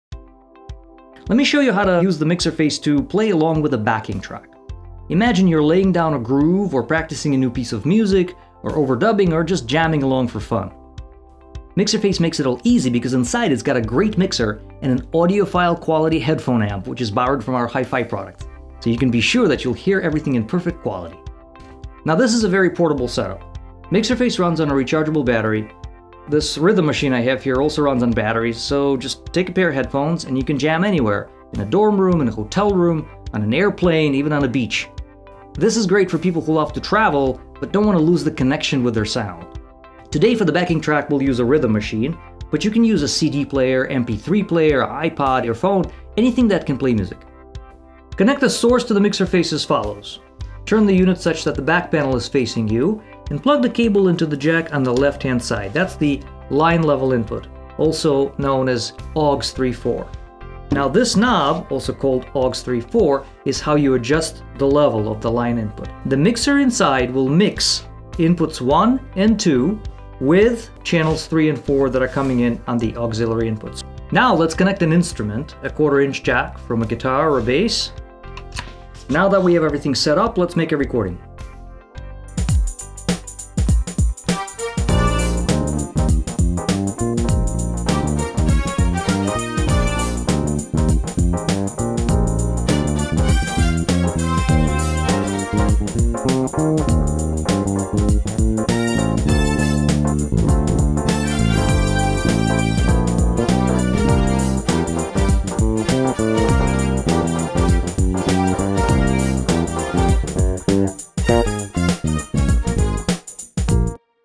Overdub and Monitor with MixerFace R4 and your iPhone
Quick Start to multi track overdubbing using the MixerFace R4 by CEntrance. Using only a drum sequencer and a bass guitar, we show you how to connect the sources and set your recording and monitoring levels to help you get the most out of your recordings. All you need is this interface and (in this case) an iphone to record and get the recording studio quality with the portability modern technology demands.